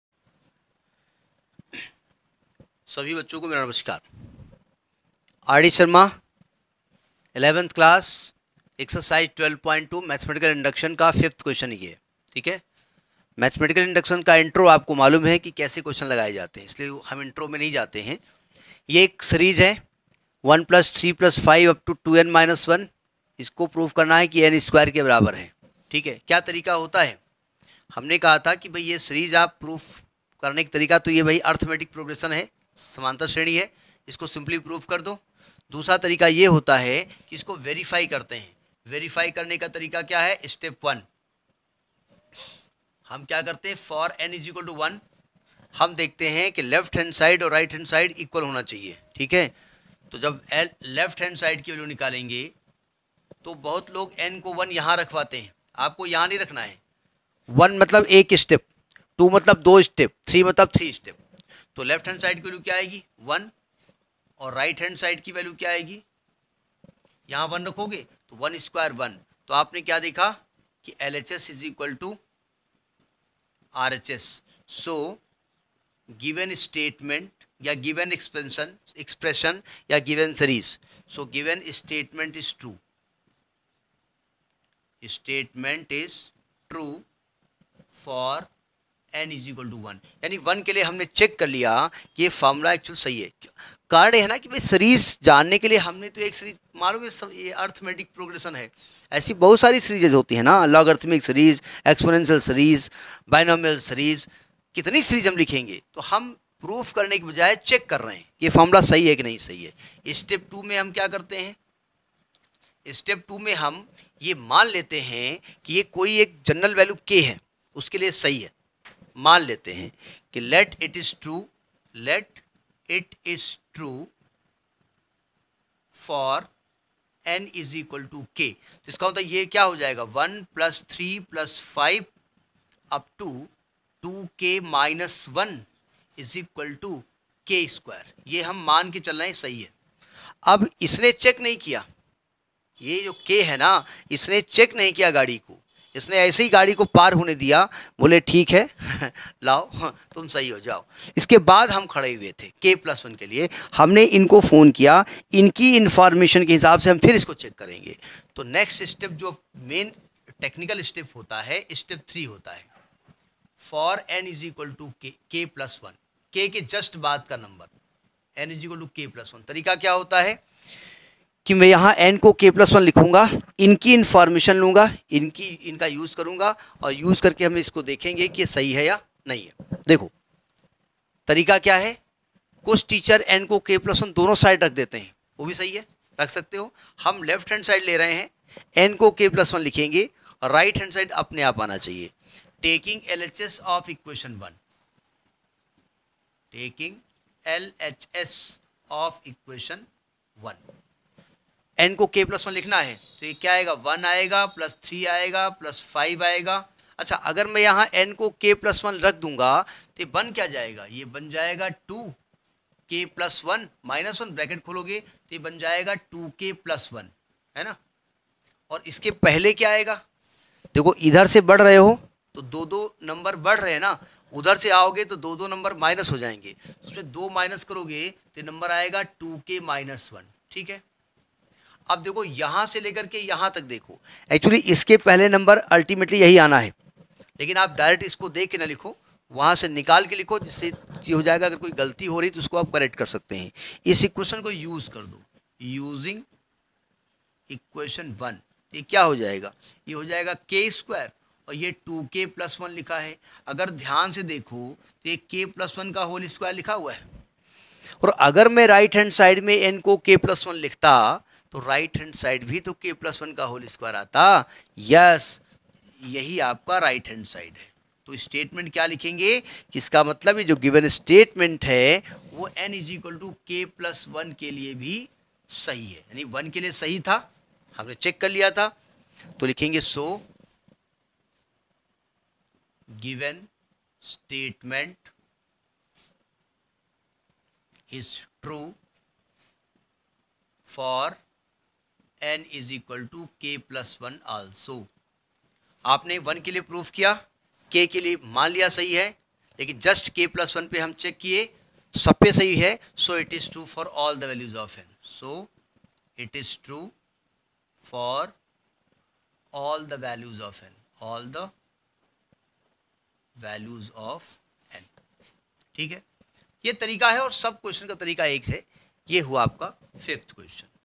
XI- Maths, R.D.Sharma: Ex.12.2, Mathematical Induction. Question no. ( 5 ). By- Atma Academy.
In this video I have discussed the Solution of R.D.SHARMA Solution of chapter 12 , Mathematical Induction.
I have explained question no. 5.